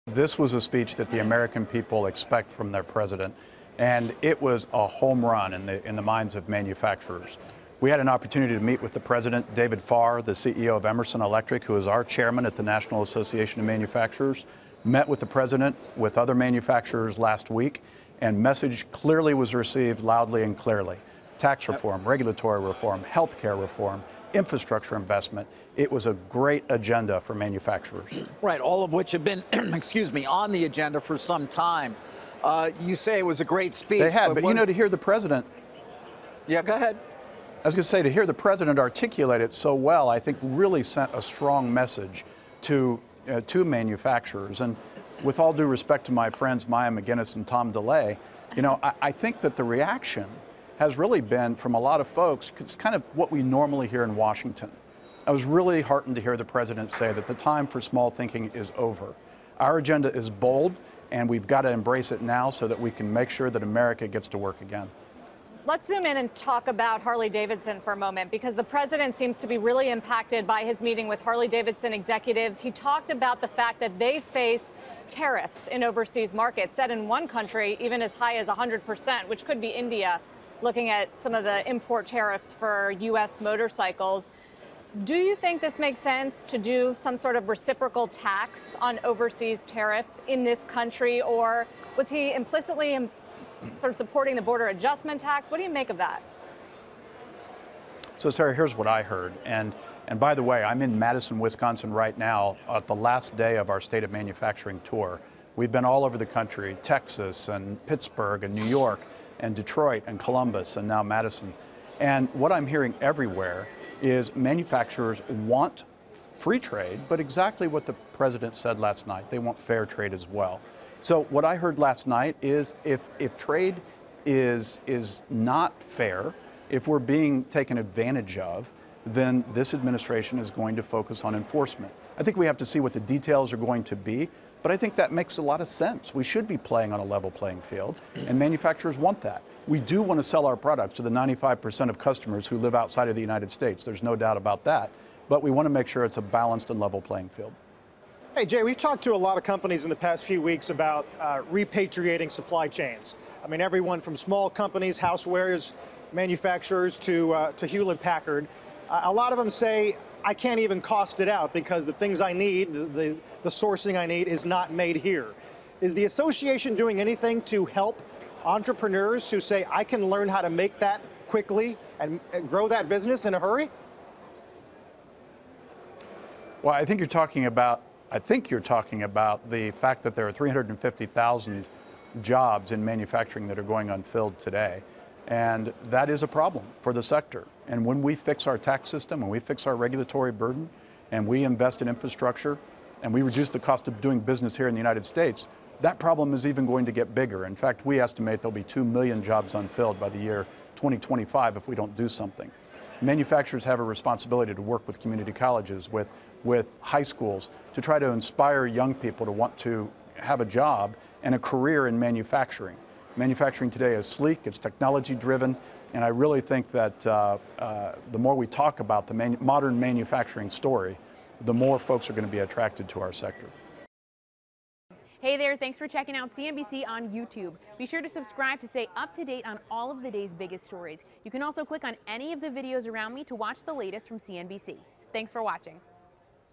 NAM CEO: President Donald Trump Speech A 'Homerun' For Manufacturers | CNBC
Jay Timmons, National Association of Manufacturers president & CEO, discusses the sentiment among manufacturers following President Trump's speech to Congress.